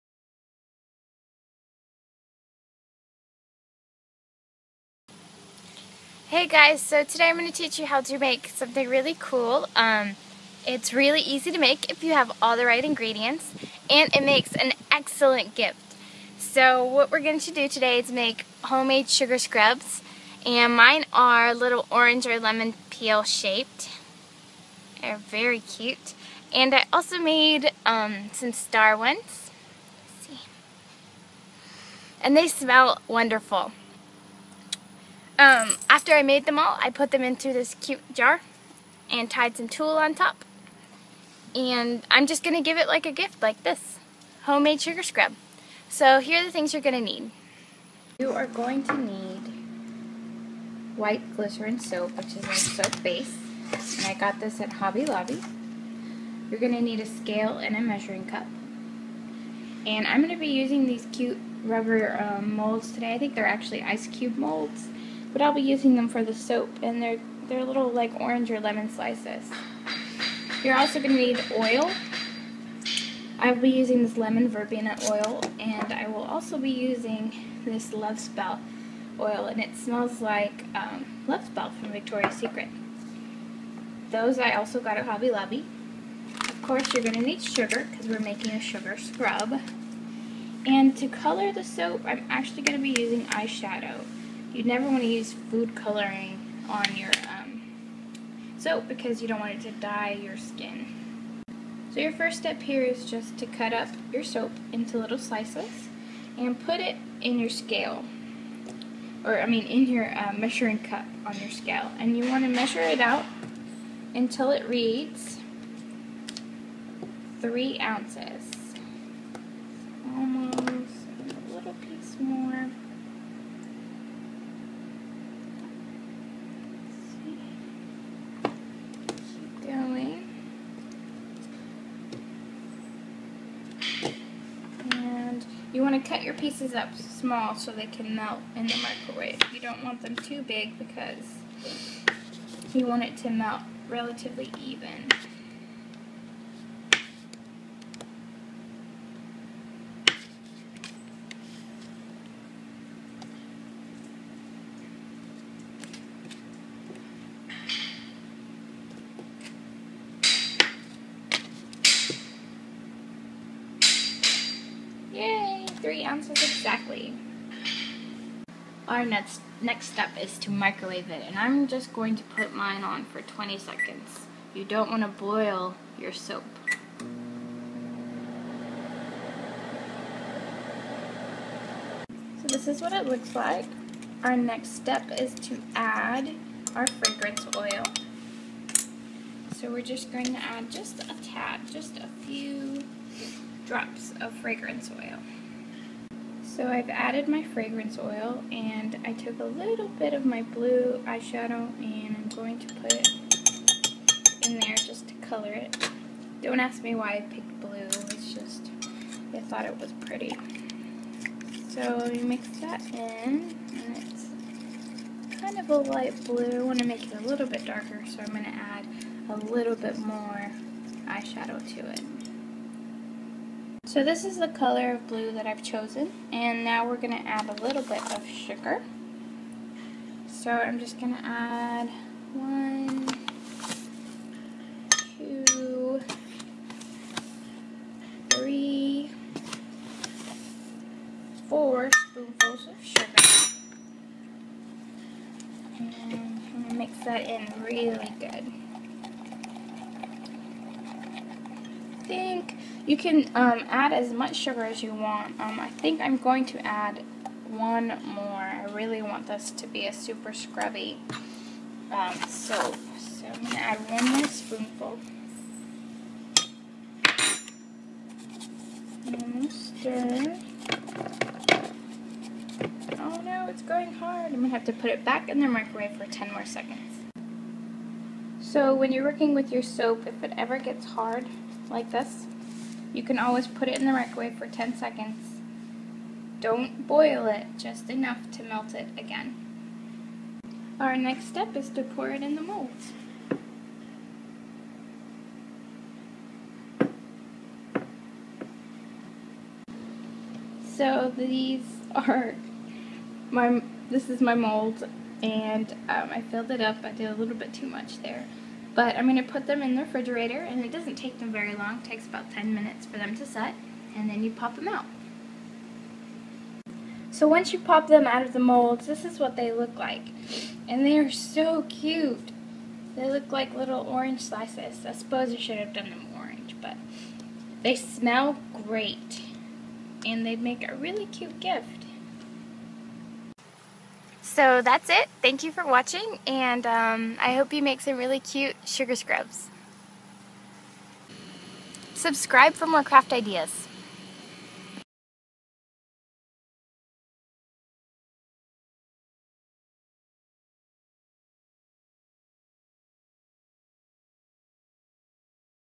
DIY Sugar Scrub MINI Soaps! (using ice cube trays)
This is a tutorial on how to make cute mini sugar scrub soaps.
MAKE your own kind and give them as gifts! GET CREATIVEEEE